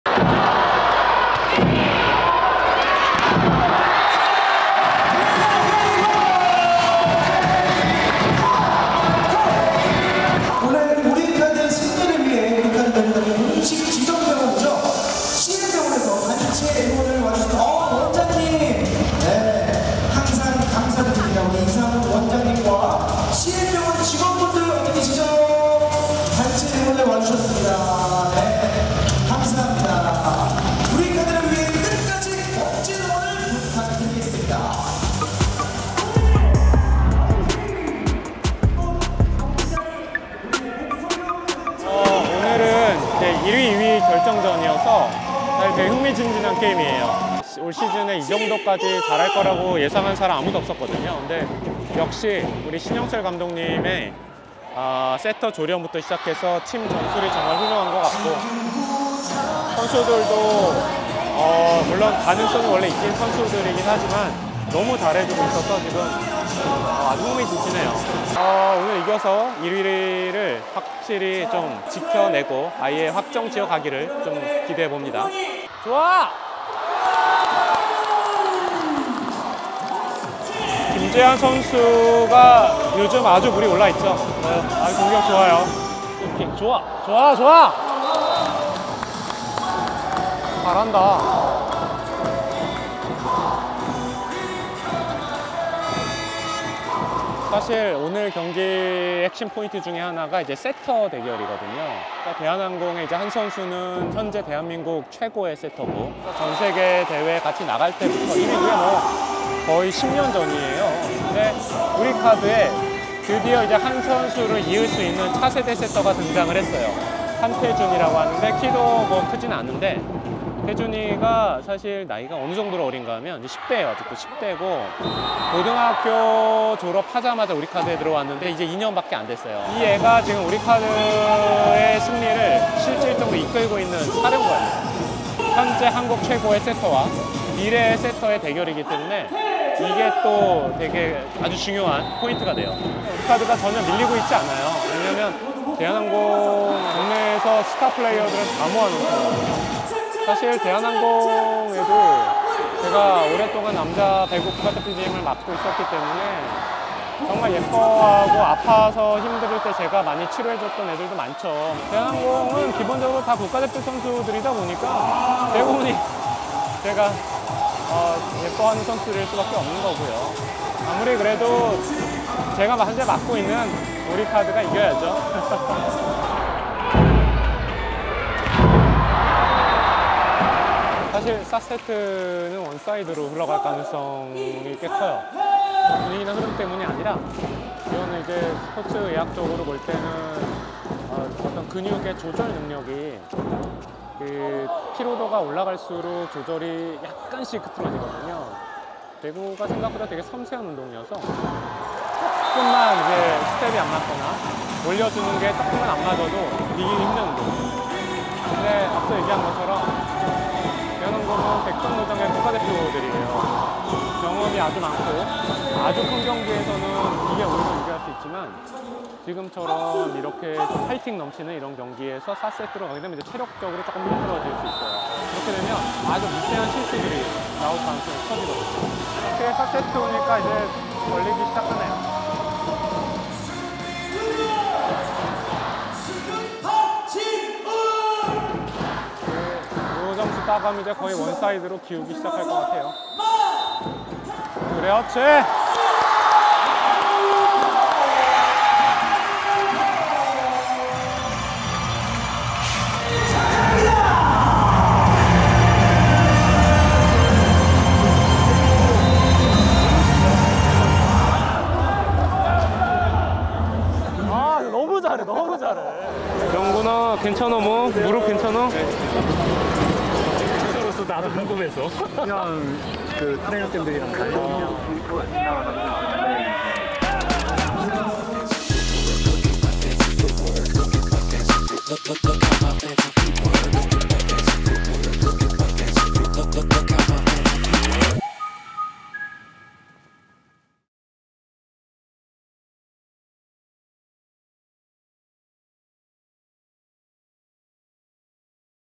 국대 팀닥터 짬바_이상훈원장이 배구 보는 자세
국가 인증 관절전문병원, 종합병원 CM병원은 국가대표 선수촌 지정병원'입니다. 국가대표 팀닥터 출신으로 여러 프로 스포츠 구단의 팀닥터로 활동중인 이상훈 병원장! 오늘은 우리카드WON배구단을 찾았습니다!